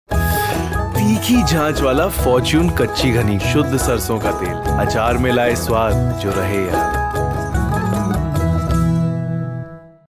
Achaar ka unforgettable taste – Fortune KGMO
Summers mein achaar nahi khaaya toh kya kiya? Aur agar achaar Fortune KGMO se bana ho, toh uska swaad hi kuch aur hota hai! Iss baar Fortune KGMO ke saath apna favourite achaar banaye aur uske lajawab taste se apni family ka dil jeet le!